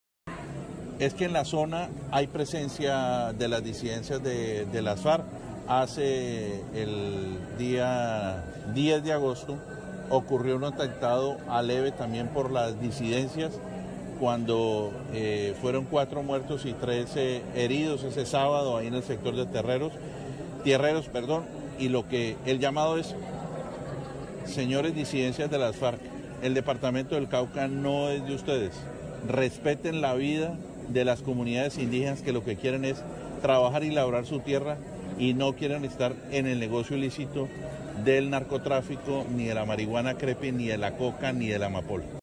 Defensor del Pueblo, Carlos Negret
Defensor del Pueblo, Carlos Negret, habla sobre lo ocurrido en Tacueyó (Cauca) donde cinco personas fueron asesinadas (una autoridad neehwe'sx y cuatro guardias indígenas) hacia las 4:00 p.m. de este martes, 29 de octubre. Hace un llamado a respetar la vida de las comunidades indígenas.